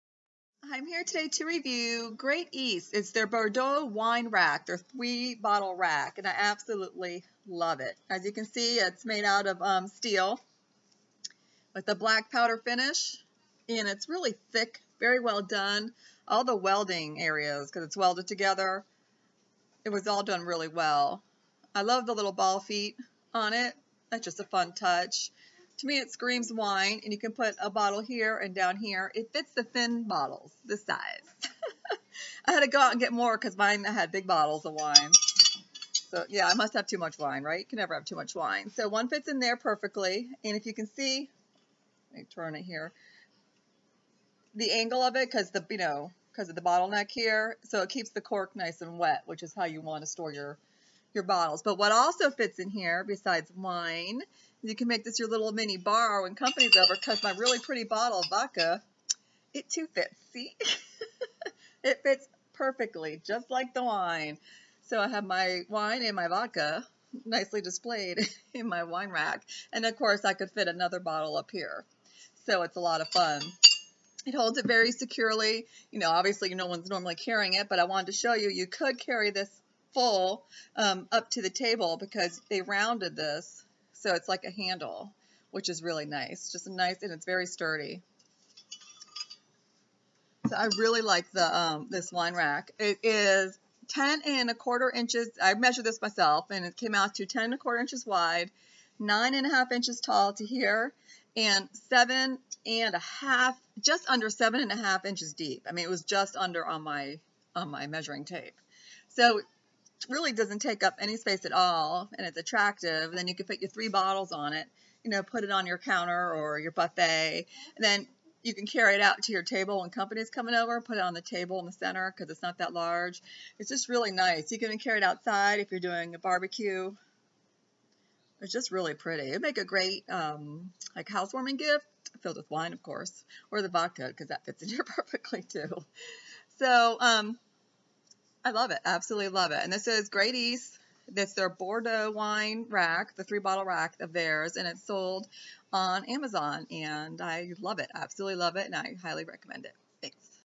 GreatEast's Bordeaux Wine Rack, 3 Bottle Rack
GreatEast's Bordeaux Wine Rack, 3 Bottle Rack. Made out of steel, with a black powder finish. Welded really well. Very sturdy! 10 1/4" wide x 9 1/2" tall x just under 7 1/2" deep. Sold on Amazon. Fits regular sized wine bottles as well as some liquor bottles.
No one has the right to my video in an way, thanks.